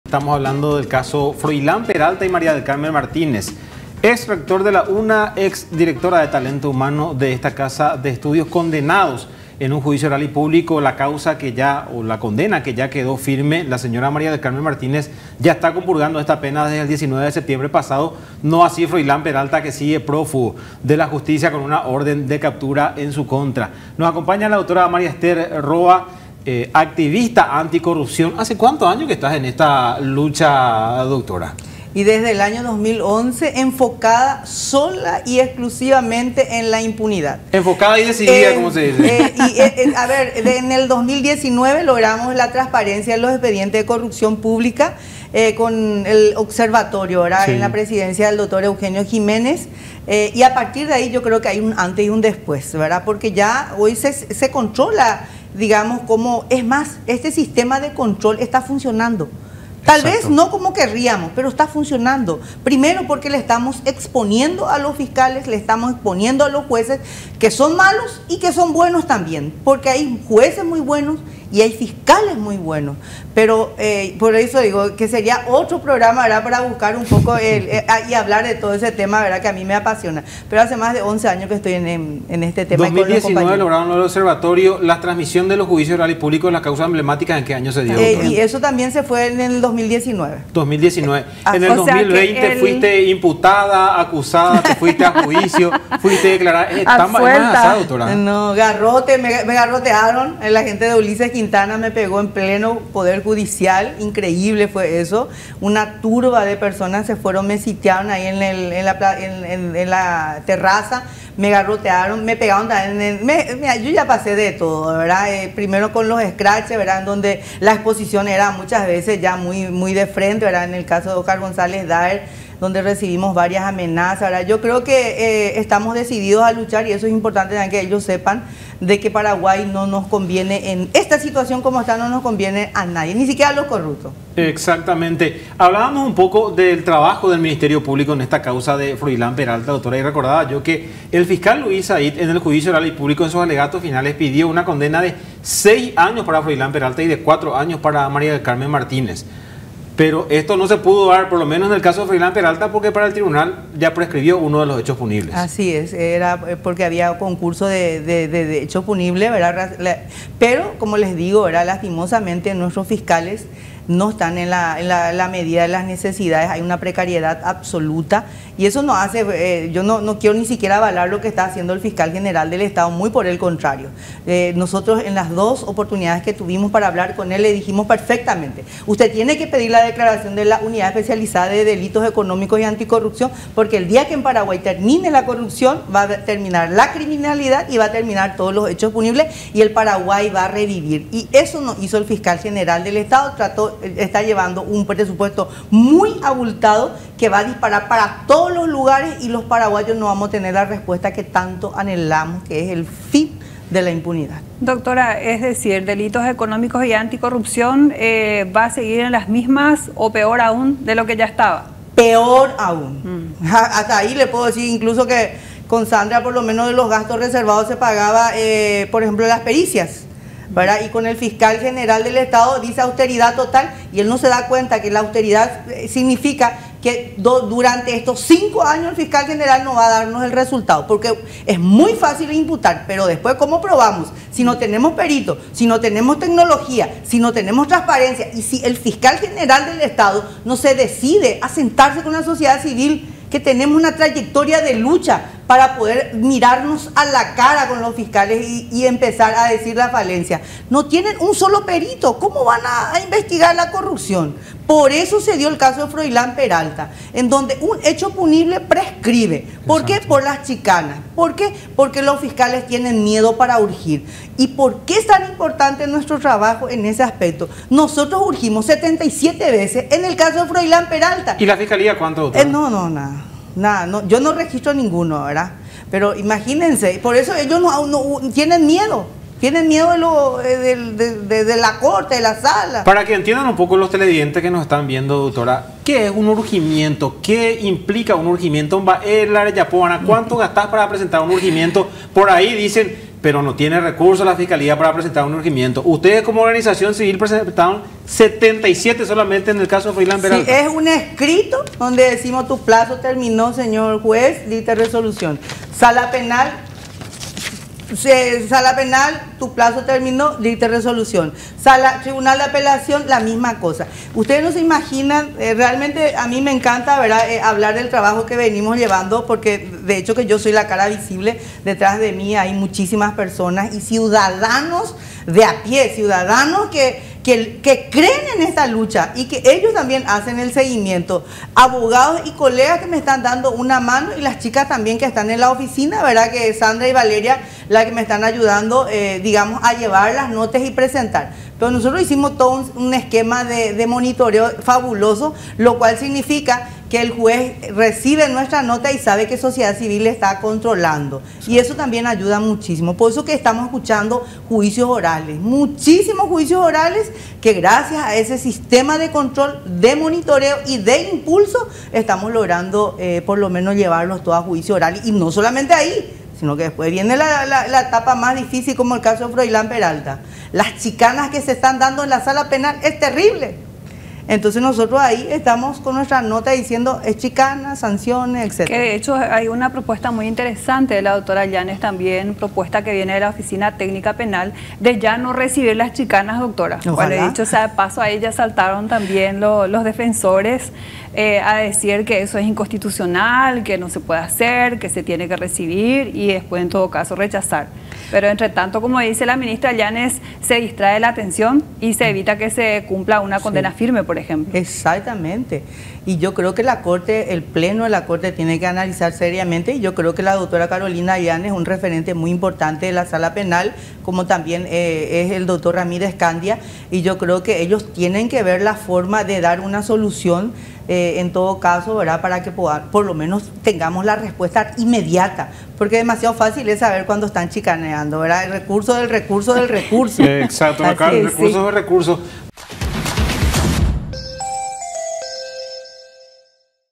“UNA No Te Calles”, 8 años después: la ‘’comepapeles’’ en prisión, Froilán Peralta Prófugo
La exdirectora de Talento Humano del Rectorado María del Carmen Martínez había sido condenada a 3 años y medio de prisión al hallarla culpable de haber contratado a personal para su granja, pero pagado con rubros del Rectorado. El daño patrimonial que produjo Froilán con su conducta dolosa en el periodo señalado de 2012 a 2015, por medio de los nombramientos fraudulentos, fue de G. 1.257 millones, aproximadamente, refiere el Ministerio Público.